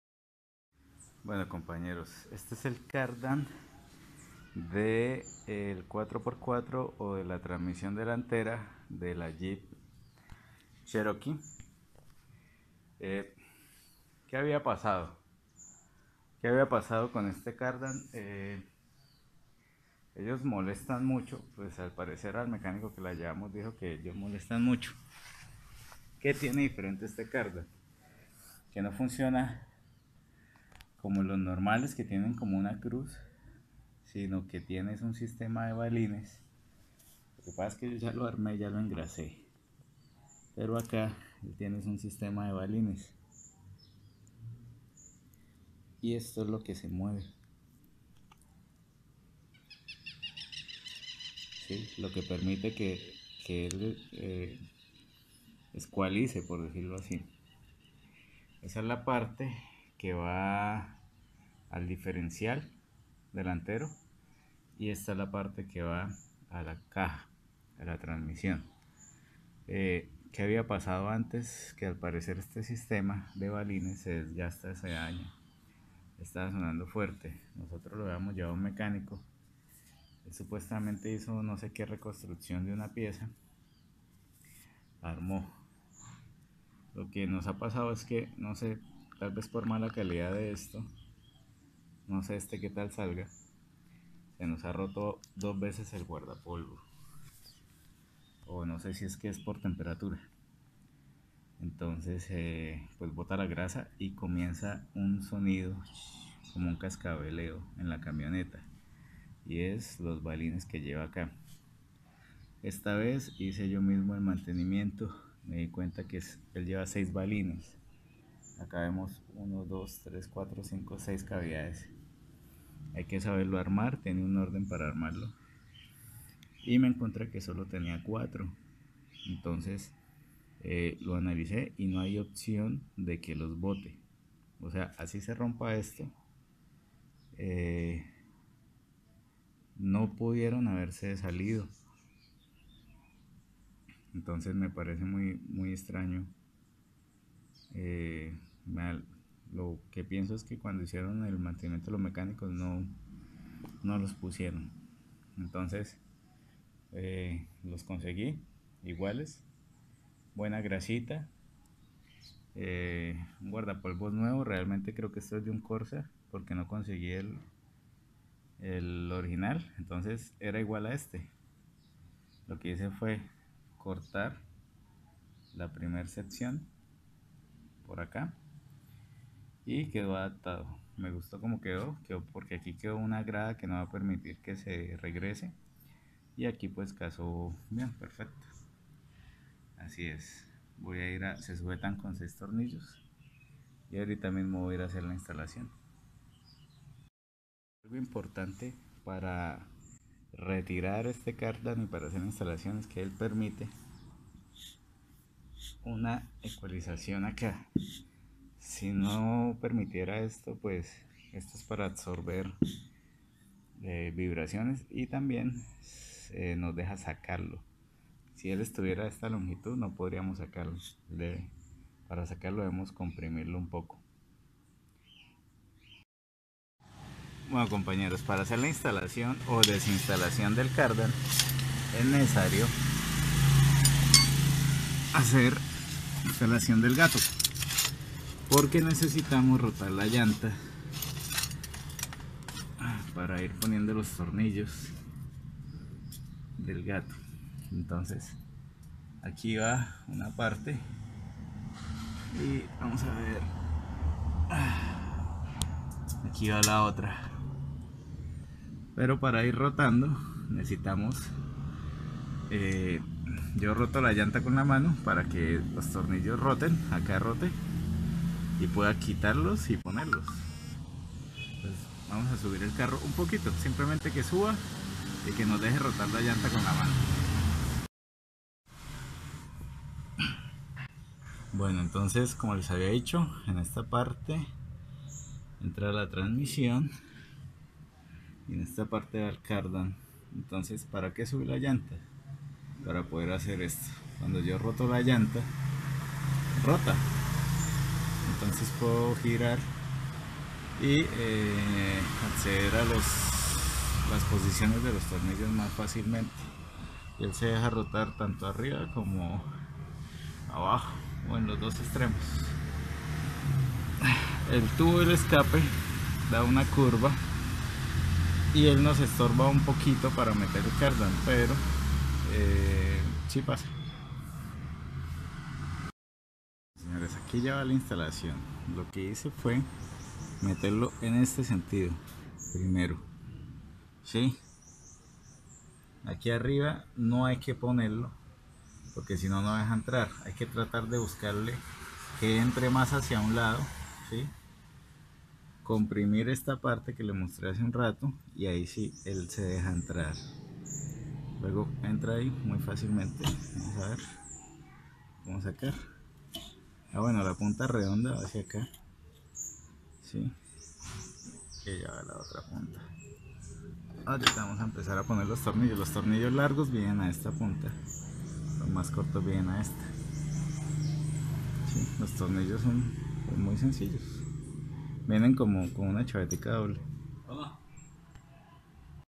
EJE TRANSMISION DELANTERA JEEP CHEROKEE LIBERTY
Se muestra el eje delantero ya que lo bajo para cambiar un empaque que se rompe seguido...